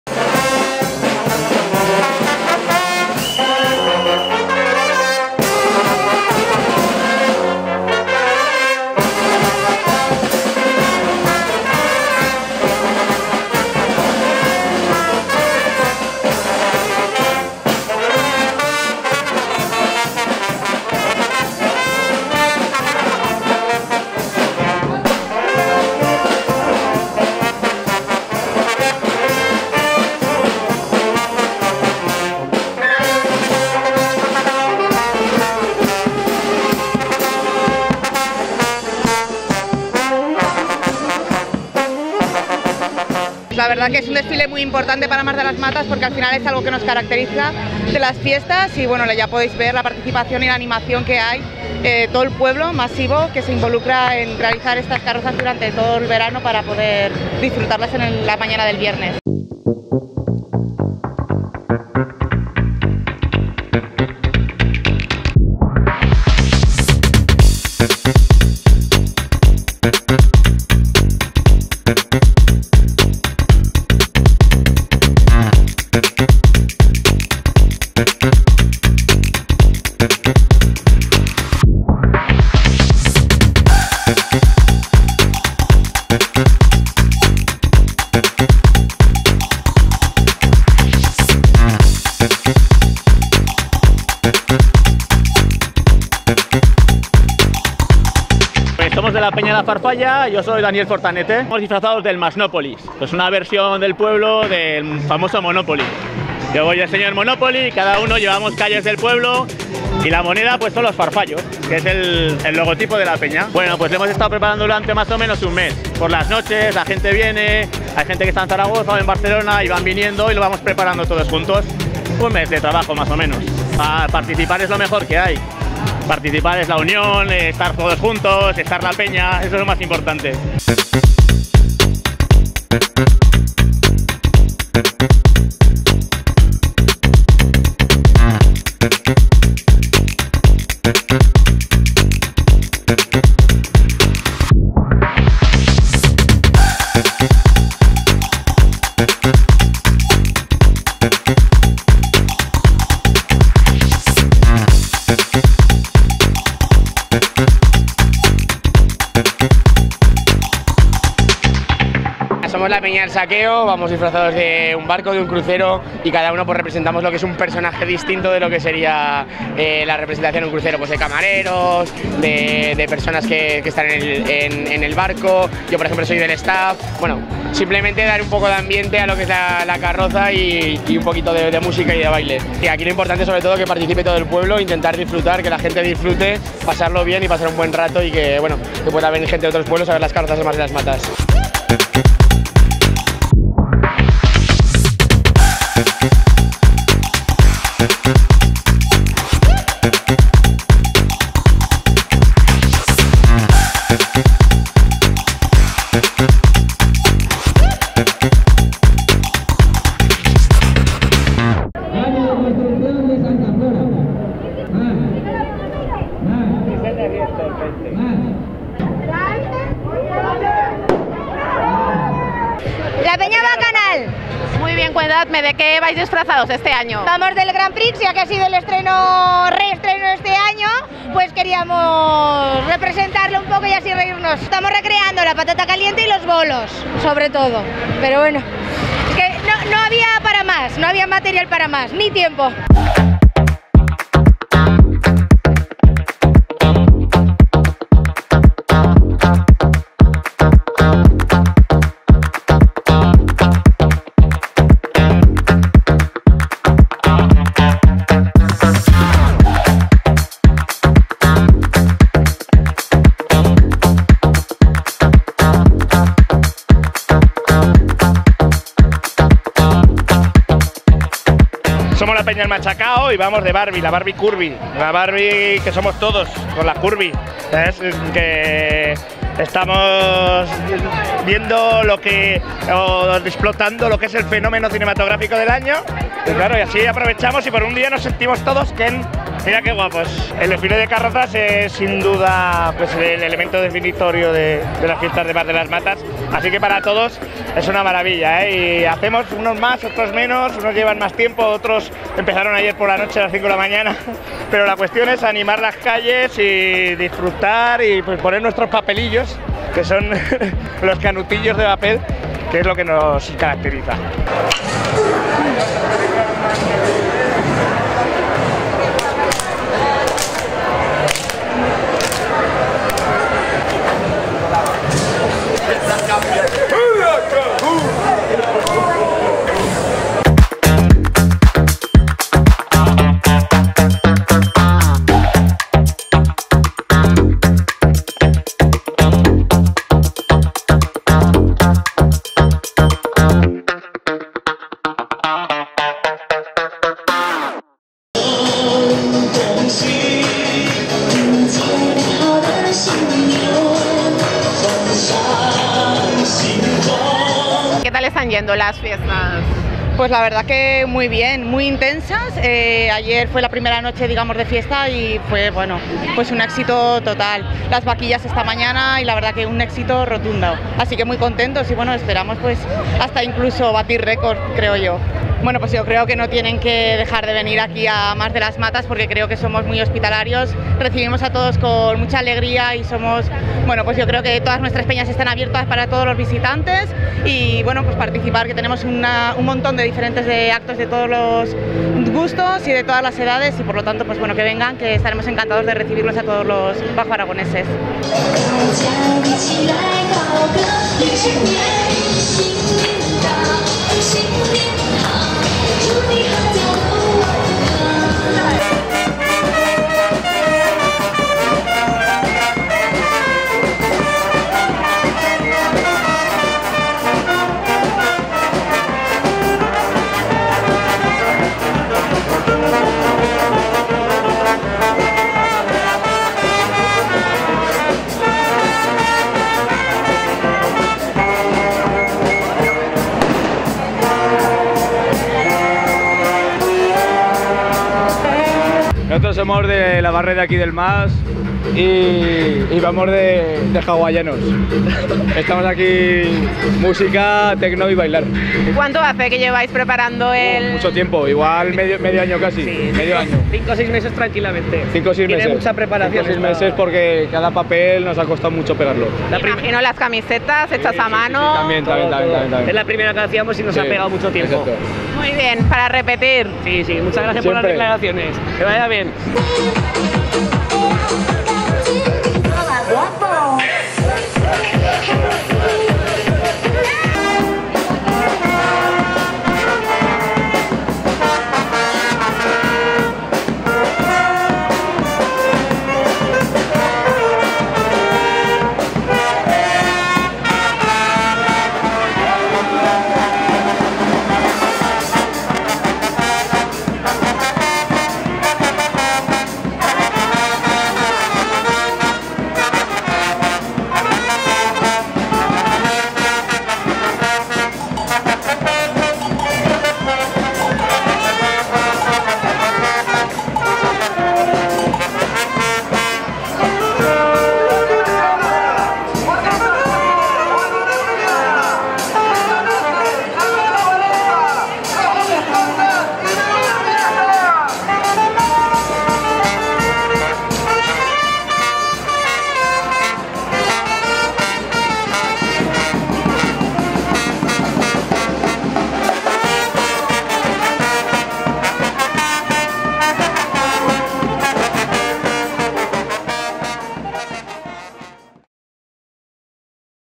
Desfile de CARROZAS | Mas de las Matas 2023
El tradicional desfile de carrozas y comparsas que se celebra en Mas de las Matas en sus fiestas de agosto, desafió al tremendo calor y llenó de color y sonrisas todo el recorrido. Abrieron el desfile las majorettes y tras la charanga fueron rockeros, un Monopoly muy masino, avestruces,  turistas en un crucero, recortables, el Grand Prix, latas de cerveza, barbies, tribus, chinos, hawaianos, marcianos y las majicas y majas de fiestas. Un espectáculo que reunió a cientos de visitantes y que su alcaldesa, María Ariño, calificó de récord de asistencia.